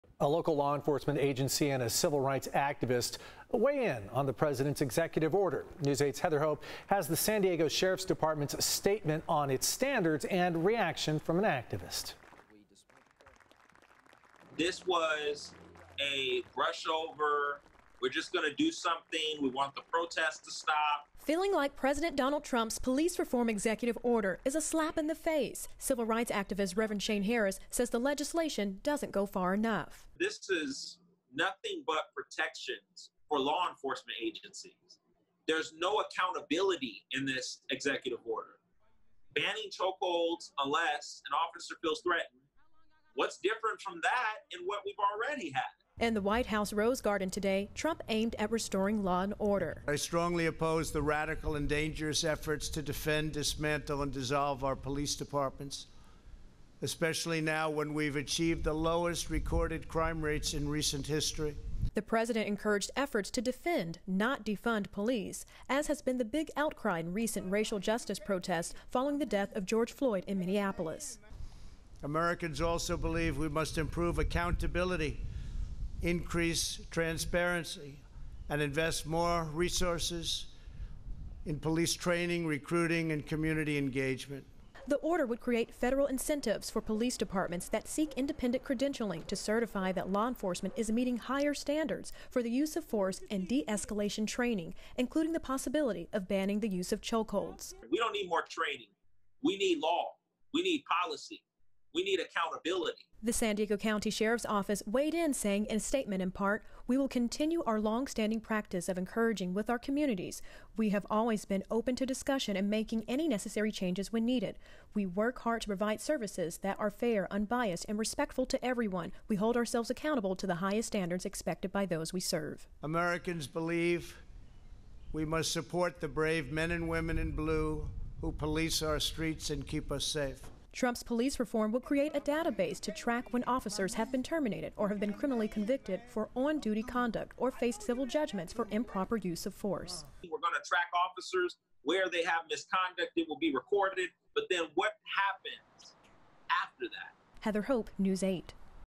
San Diegans react to police reform executive order
While some approved of the president's move, others say the order doesn't come with strong policy for sustainable change.